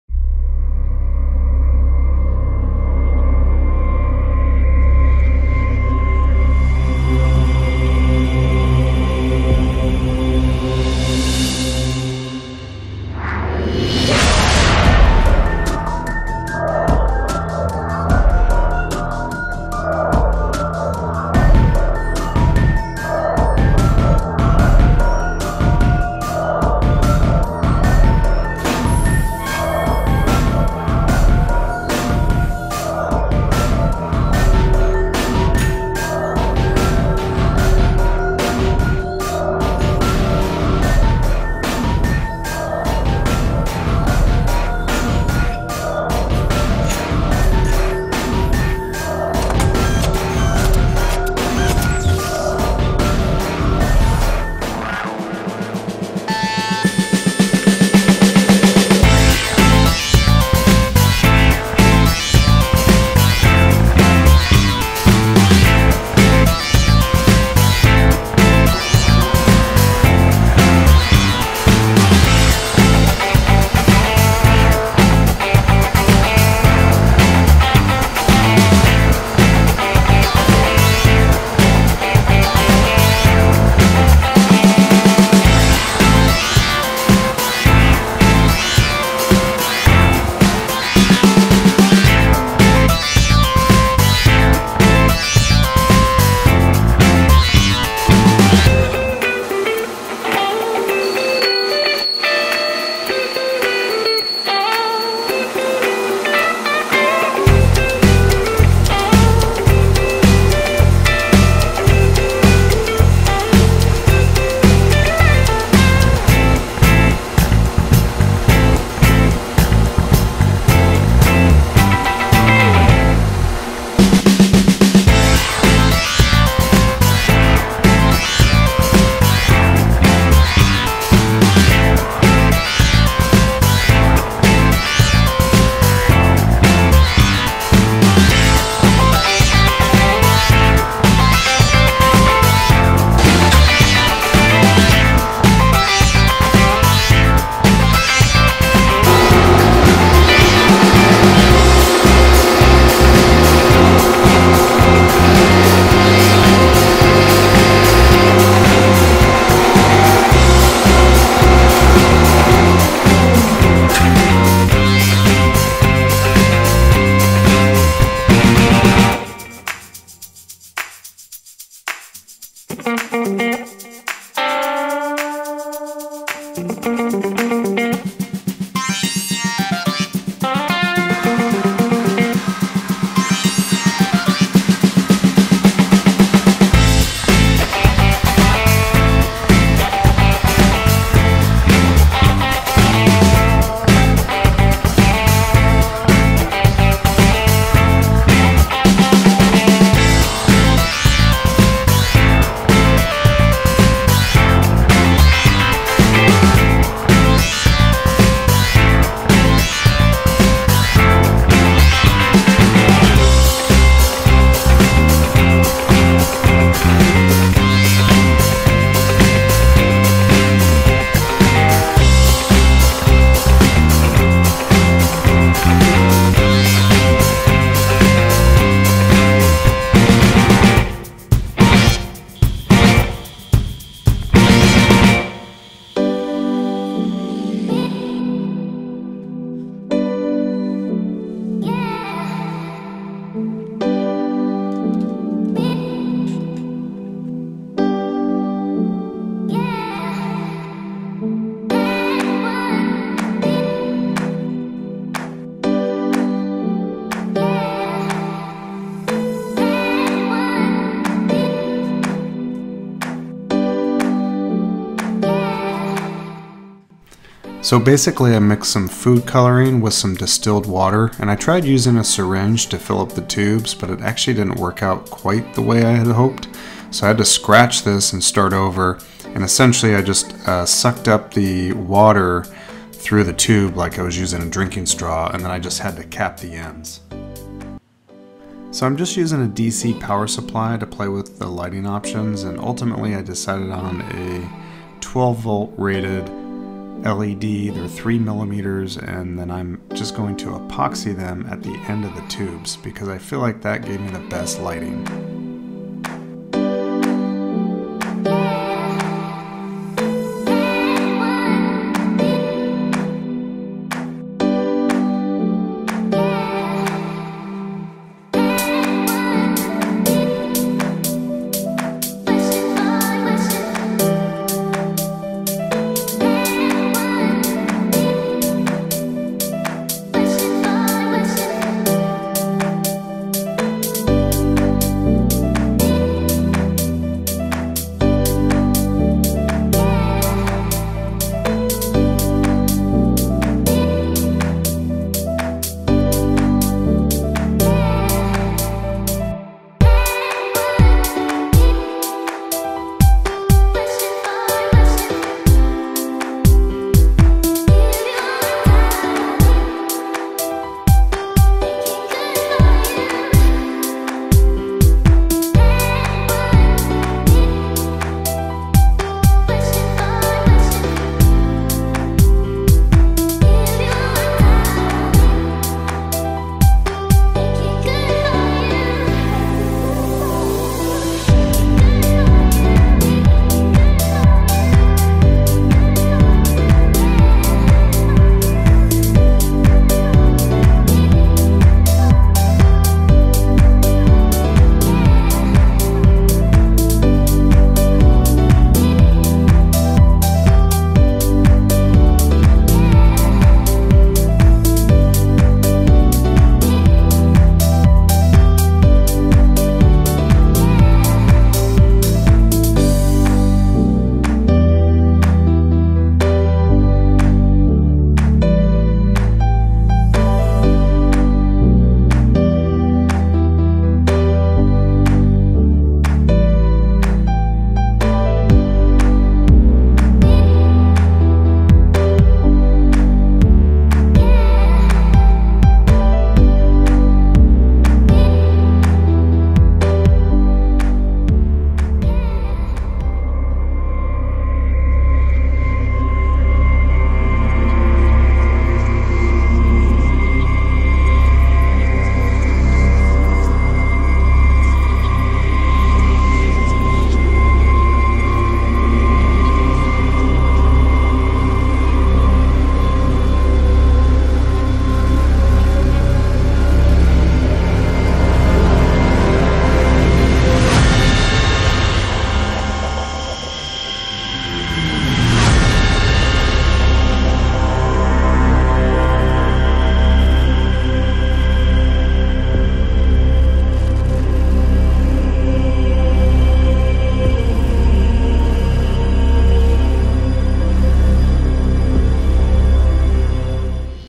How to Make a Resident Evil T-Virus Canister
In this video I show you how I made a T-Virus canister from the movie Resident Evil.  This is definitely an extreme build, so if you don't have the machinery I do, be creative and try using PVC fittings--which you can paint with a chrome or aluminum paint.